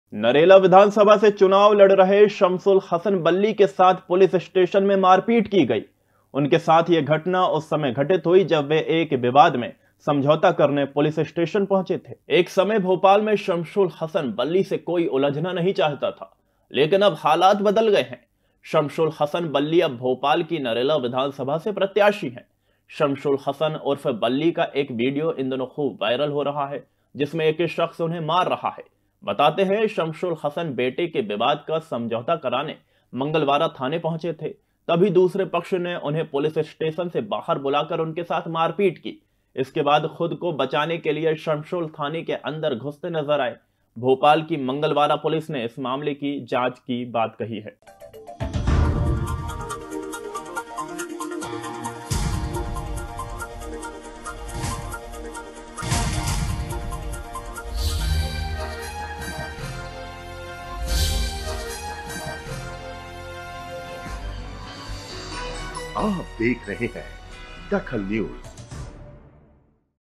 DAKHAL NEWS शमशुल हसन बल्ली के साथ मारपीट
शमशुल हसन बल्ली के साथ मारपीट
पुलिस स्टेशन के सामने हुई वारदात